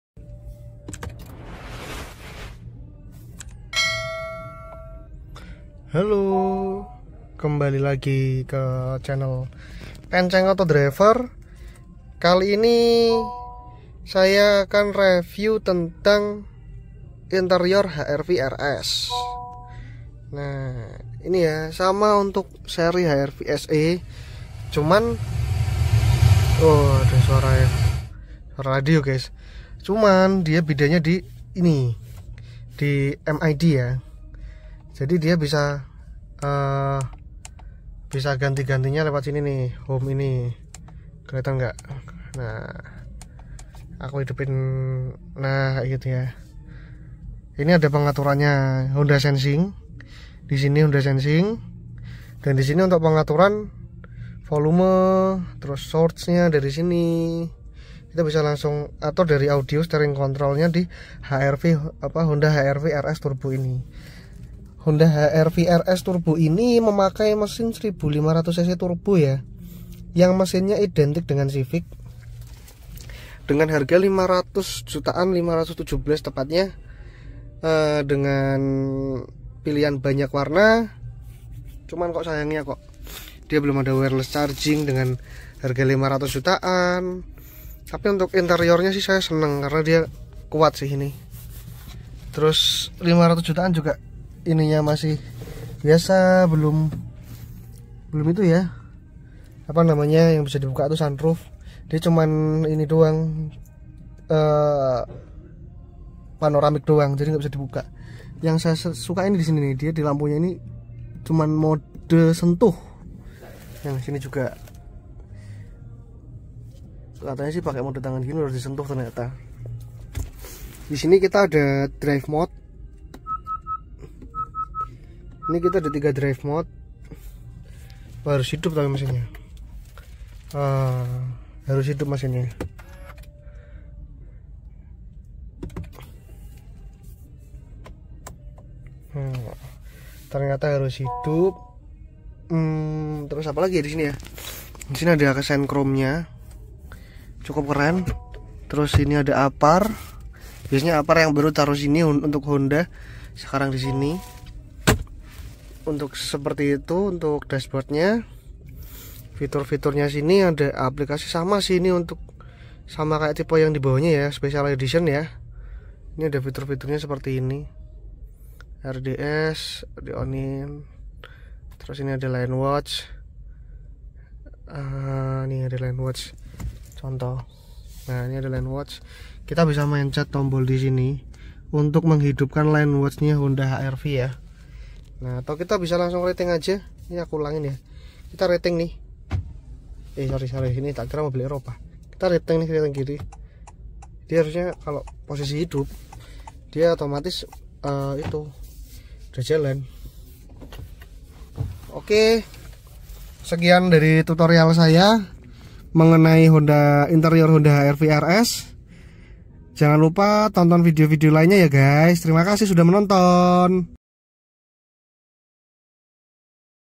HRV RS Interior Review (In depth tour)
Saya akan mereview interior hrv rs turbo dan apa aja sih bedanya dengan tipe hrv lainnya yuk disimak.
HRV RS ini juga sudah memiliki fitur honda sensing lhooo dan audio steering control nya.